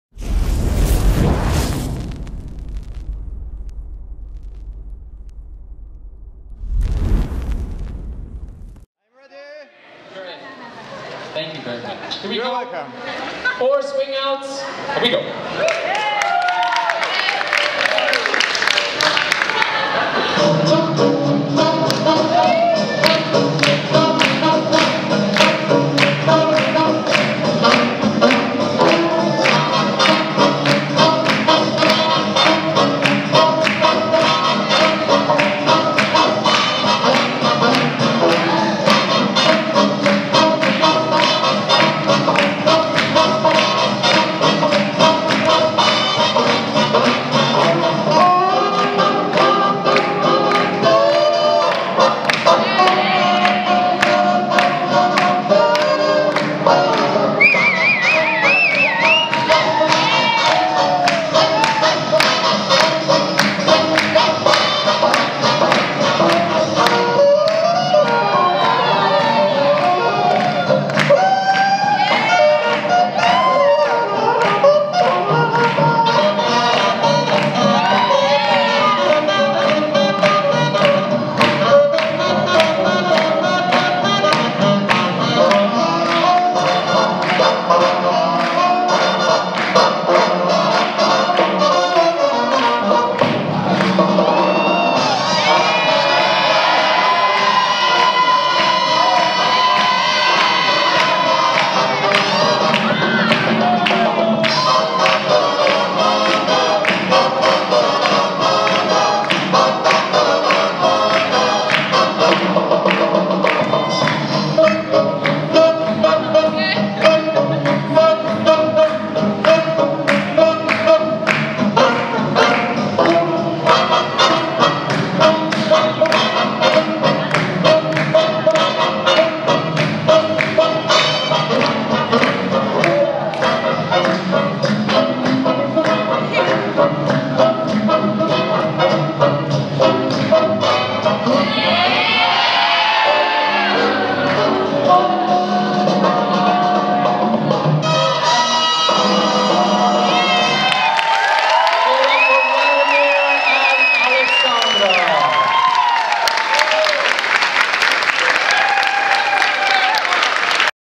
Dragon Swing 2018: Aleksandra Teslenko & Vladimir Marchenko
Performance on Sunday Party: "Four Swingouts" by Aleksandra Teslenko & Vladimir Marchenko

video by: Mateusz Wolski